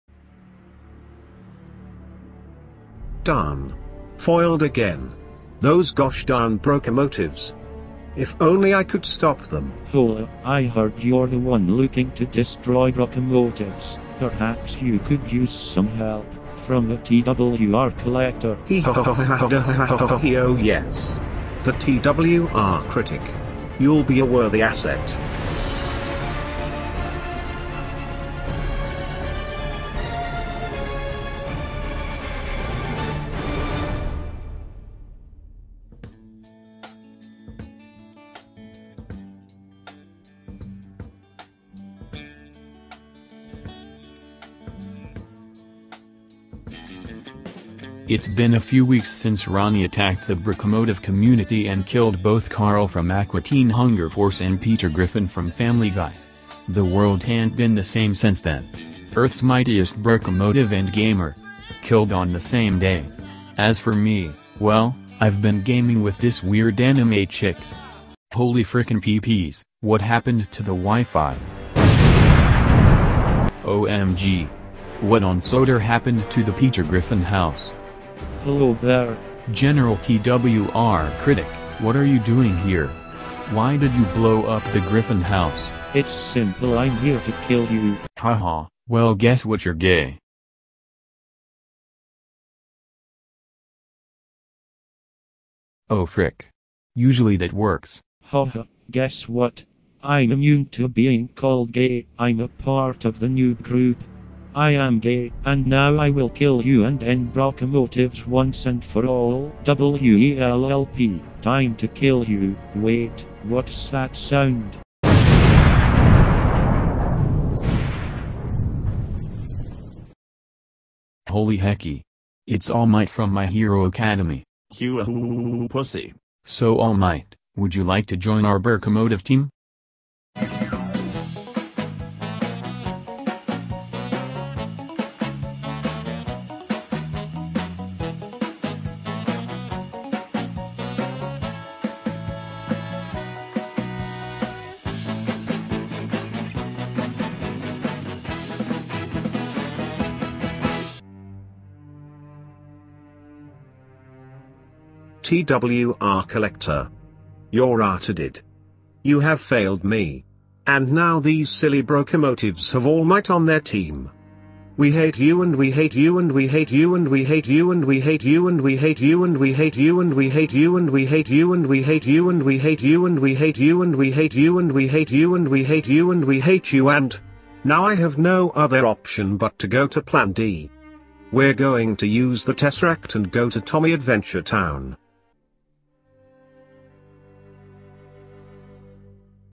Thomas and Carl: Age of TWR Collector
Bubbling Boilers! Thomas and Carl are back! This time they fight someone else. and t55productions appears!

Thumbnail by Ben11GWR!